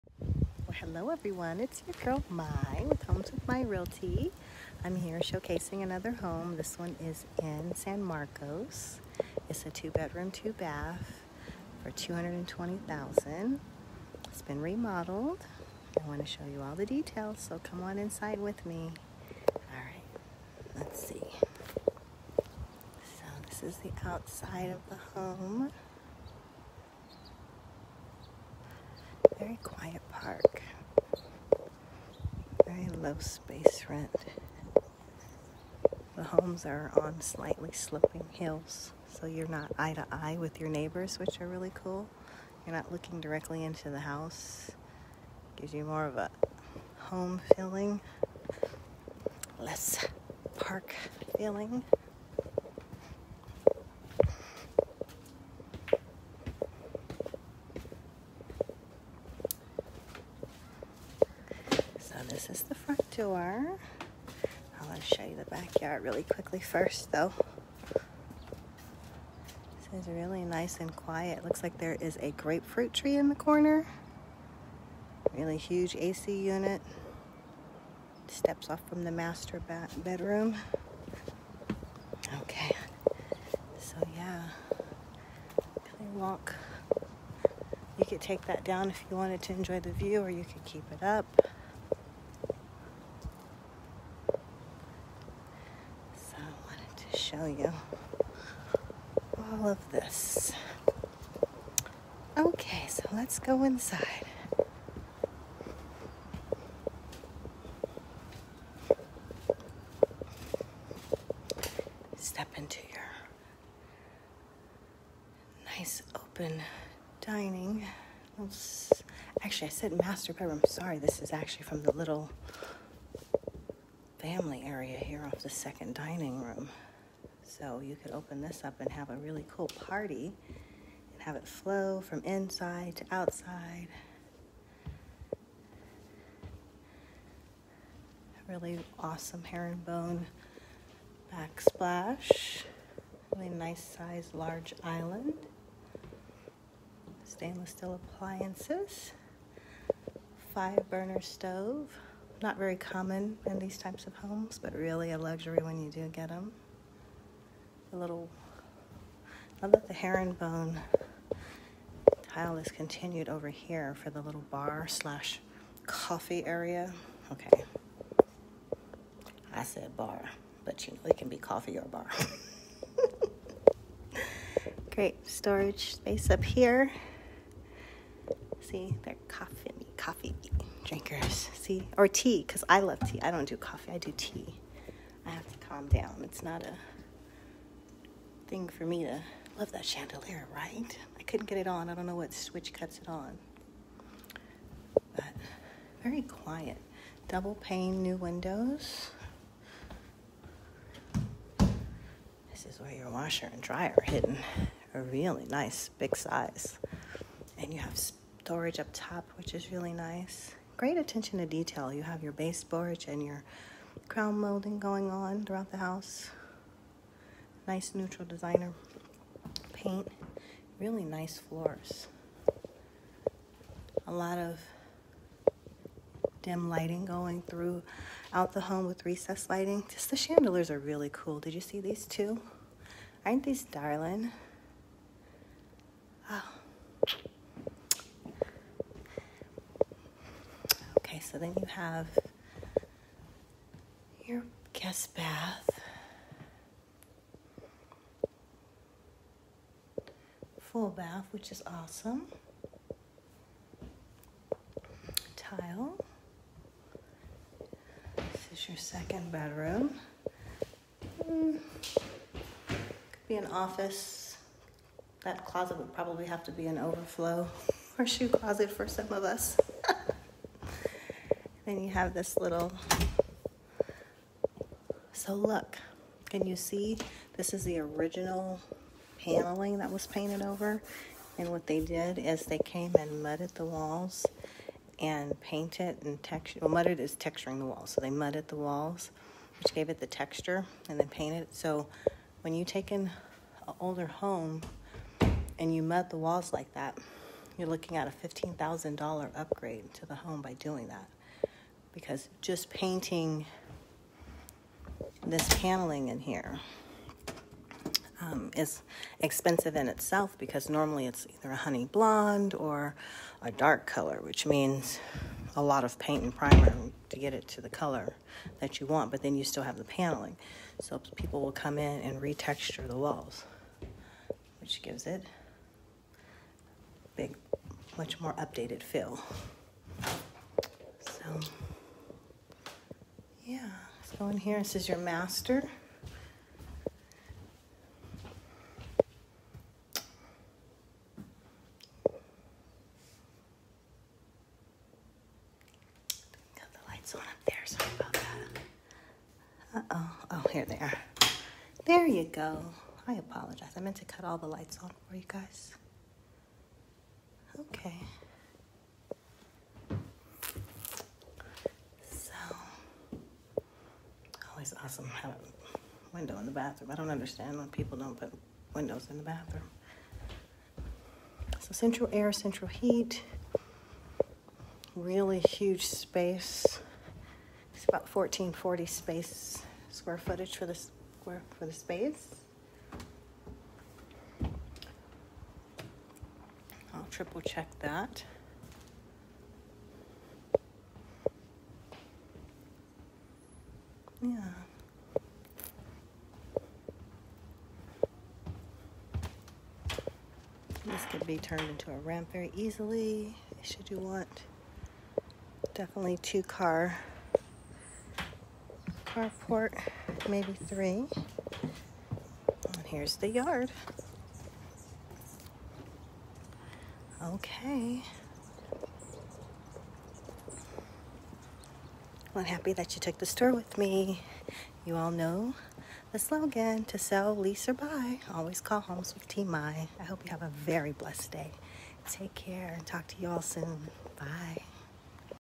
1145 E Barham Dr #205, San Marcos, CA
55+ community rental park. Move-in ready 2 bedroom in attractive community! Home has been remodeled including new drywall, laminated flooring, air conditioning, canned lighting, crown moulding and more. Offers dual-pane windows and a sliding door for easy indoor and outdoor access. Both the kitchen and bathrooms have been remodeled. Ample yard is easy to maintain. Close to freeways, shopping and more. What a beauty!